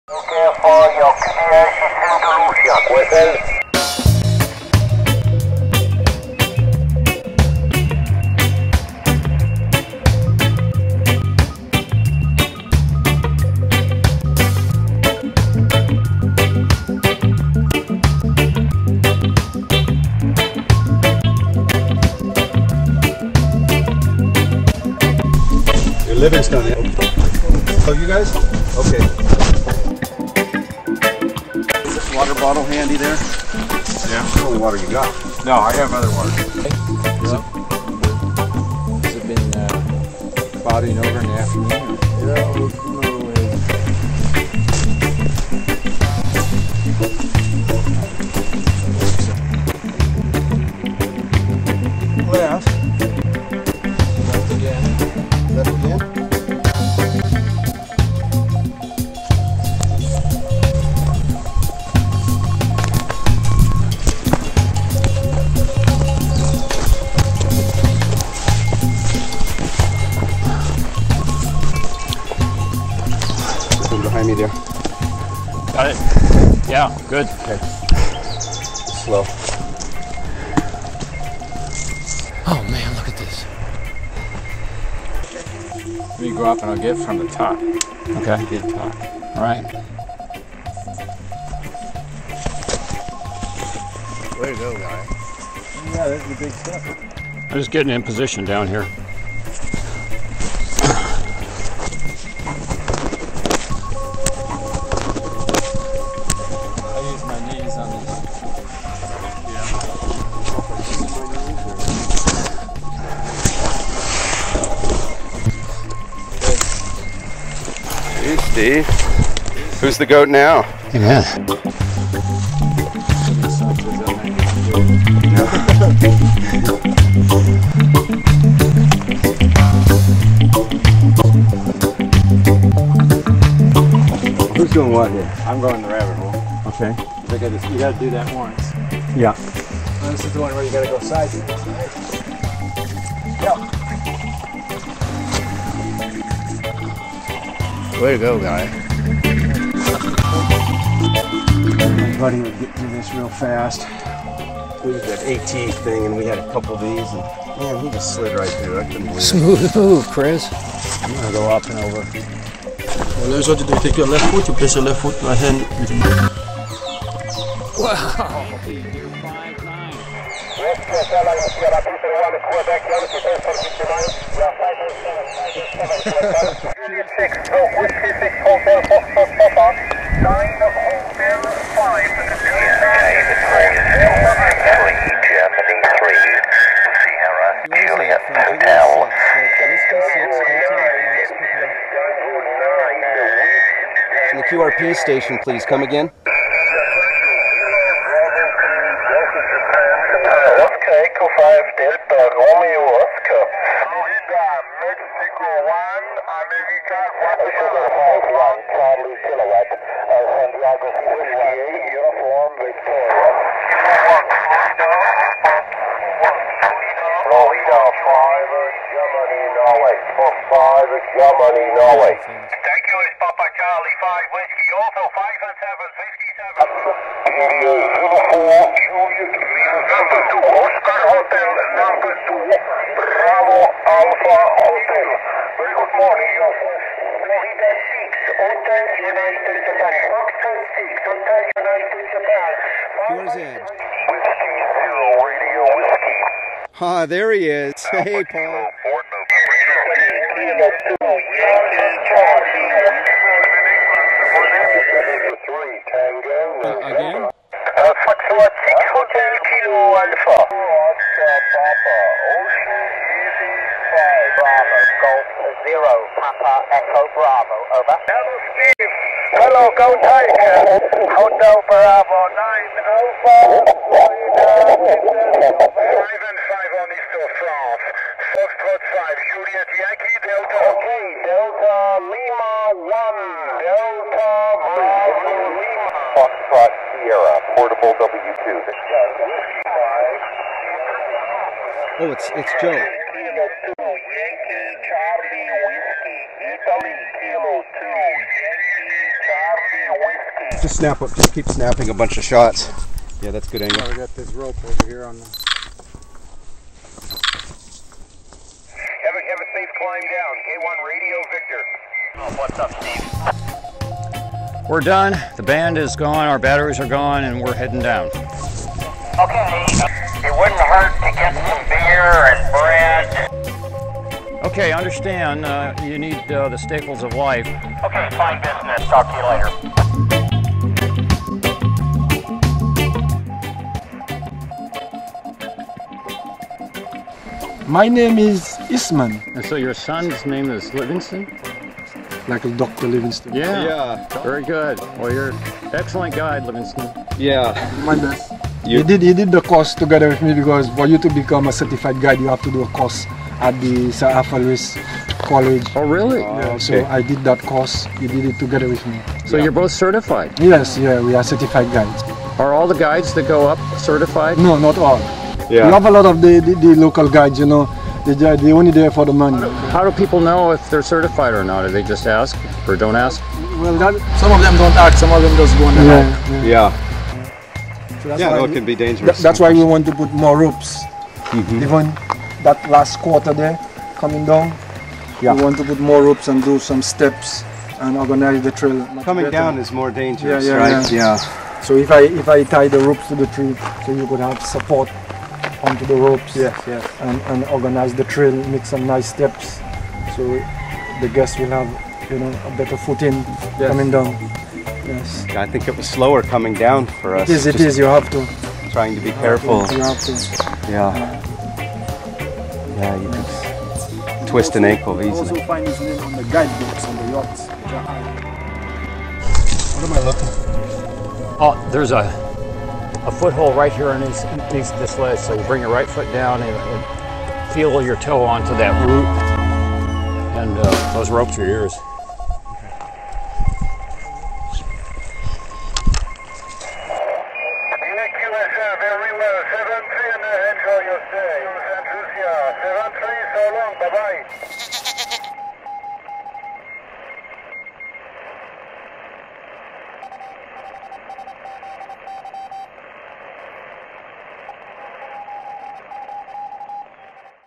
Operating from the Top of Petit Piton on St Lucia
Ham radio operators Chris- J6/W6HFP, Guy J6/N7UN and Steve J6/wG0AT climb up the remains of an ancient volcano Petit Piton (St Lucia Island) in order to put this summit on the air for the first time. Using ultralight equipment (FT857d at 50-watts, Versatee vertical and A123 battery) they make contacts world-wide from their island summit on the 17-meter band